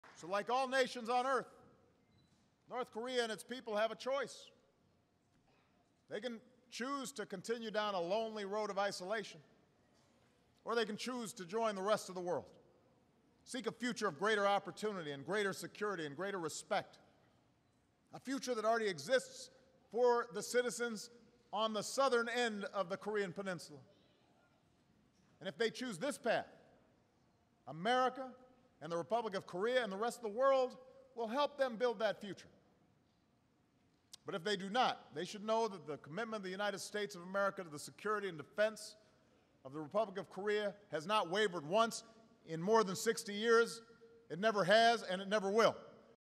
SKOREA:OBAMA SPEECH-NKOREA HAS CHOICE
President Obama speaks at Yongsan Garrison the headquarters for U.S. forces in South Korea.